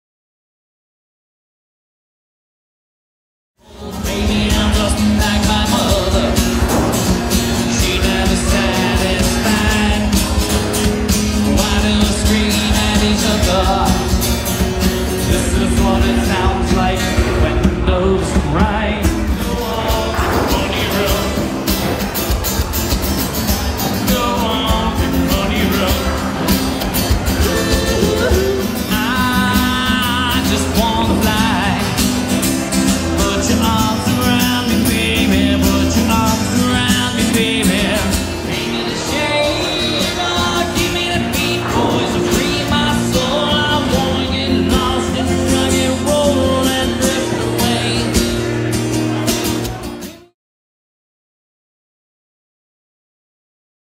Rob Samay LIVE at Foundry Social for Friday Night Live!
Rob Samay brings his awesome tunes & amazing hair back to the Foundry Social Main Stage for Friday Night Live!